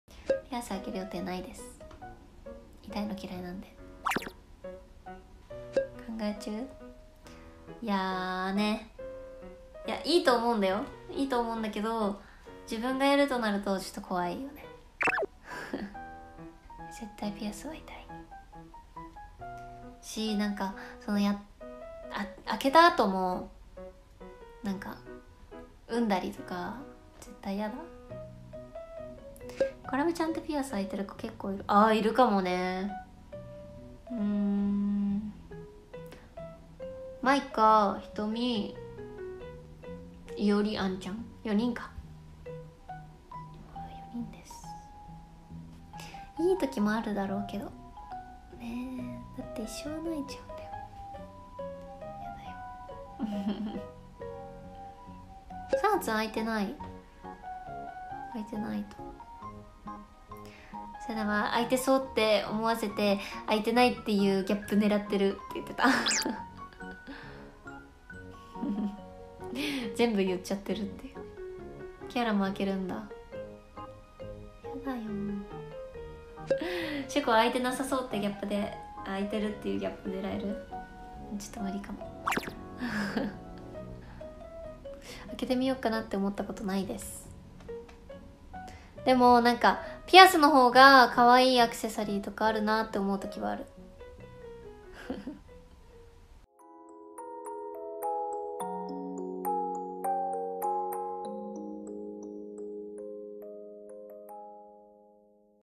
【イコラブ】メンバーのピアス事情を話す瀧脇笙古【=LOVE（イコールラブ）】
大河ドラマ狙うなら開けない方がいいかもね笑

切り抜いてほしいシーンがありましたらぜひコメントお願いします。